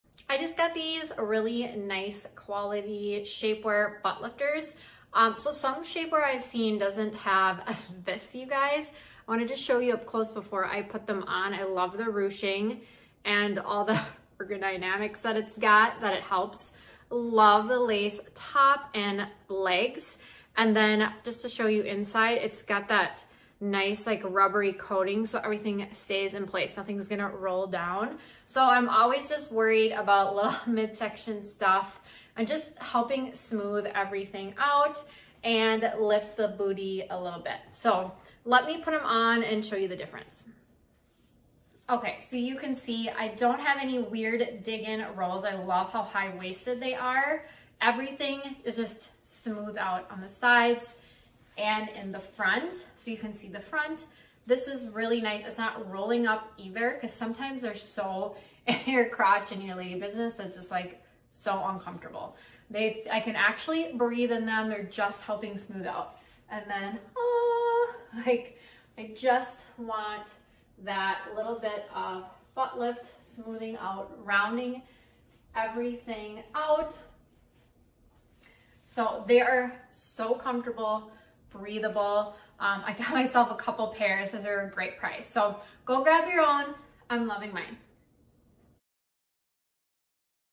Shecurve Snatched Butt-Lifting Faja Short
Achieve the butt of your dreams with our comfortable yet effective butt-lifting short. The high waisted Power net design allows for maximum compression in your tummy while sculpting your butt for an effortless and refined look never noticeable under clothes.